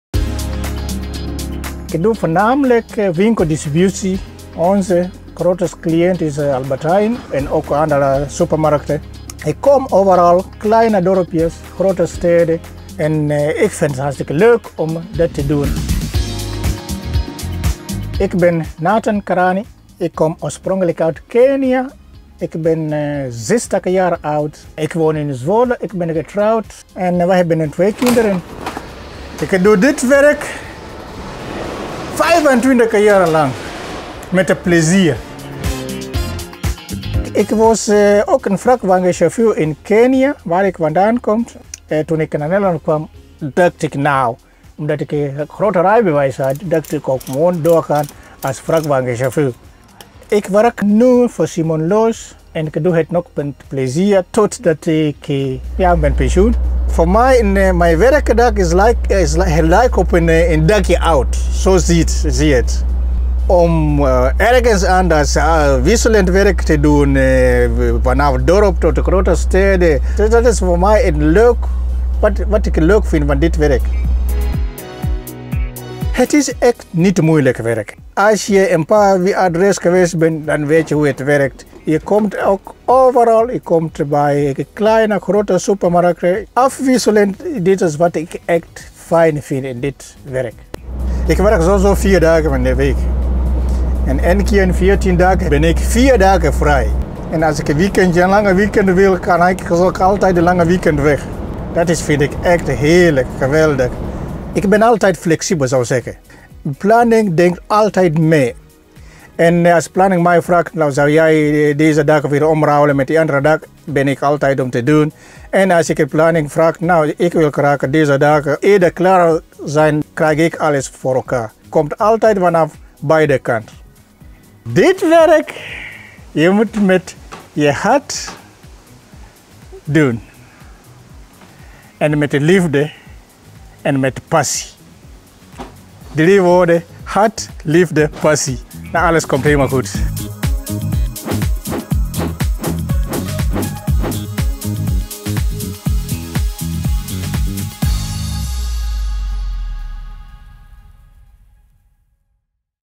Ontmoet Nathan (60): de man achter de supermarkt bevoorrading | STL.NL
De wereld van de transport en logistiek is groot en breed. Je kunt in veel verschillende functies en deelmarkten werken. Deze verschillen tussen deze functies en deelmarkten zijn groot. Zo is er voor elk wat wils. Bijvoorbeeld als chauffeur distributie bij Simon Loos.

Als chauffeur in het distributievervoer kun je direct instappen. Met de benodigde certificaten mag je al de weg op. De ideale start van jouw baan in een sector met toekomst. Meer weten?

STL Werkt. Voor Jou!